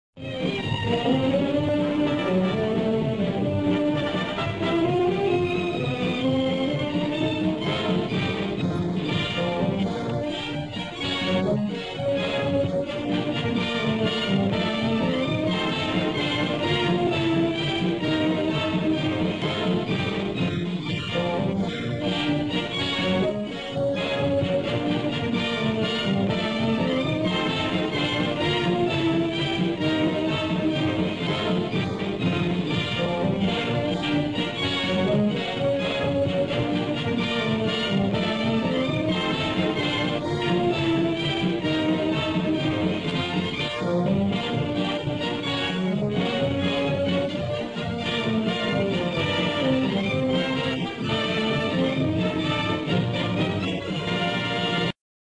Old Hunts Pier Movie Trailer Black and White
This is a black and white trailer for Hunt's Pier that played in movie theaters in Wildwood, NJ in the 1960s. You might remember riding the rides on your Wildwood vacations as a kid. The music was added, as there was no soundtrack on the original film.